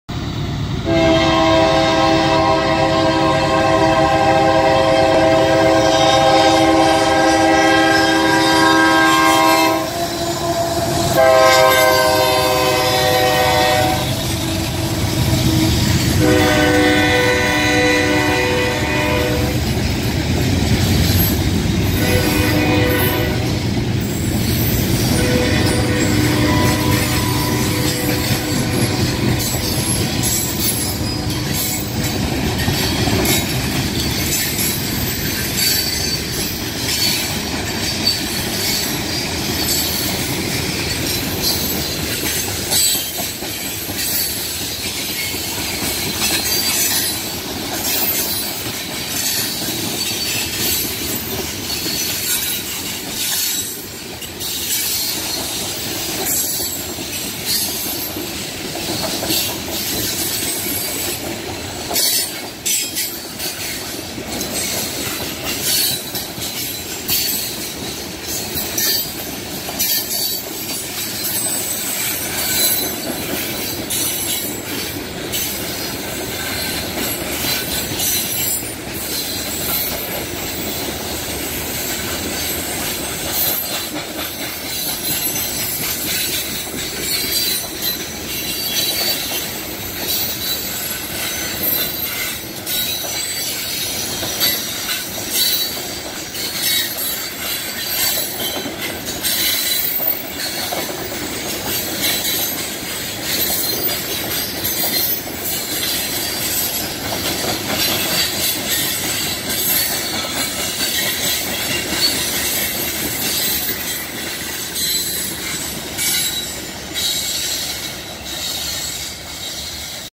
CSX W221 Eastbound with UP power
CSX loaded coke train W221 heads east through Covington, GA on 11/08/2018.

UP 5704 - GE C44AC
UP 7650 - GE C45AC GEVO

Taken with a Samsung Galaxy S9+